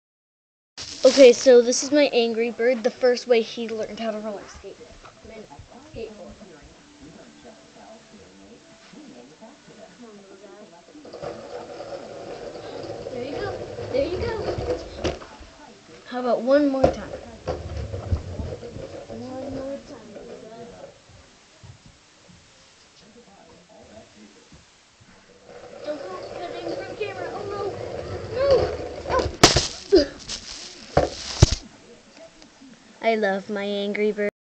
Angry bird: skateboarding!
Just teaching my new Angry bird some tricks